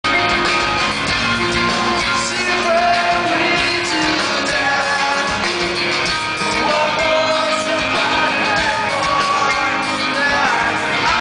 ffaf - 14th nov 2005 taste of chaos
funeral for a friend at taste of chaous with killswitch engage and the used 14th november 2005 - london brixton academy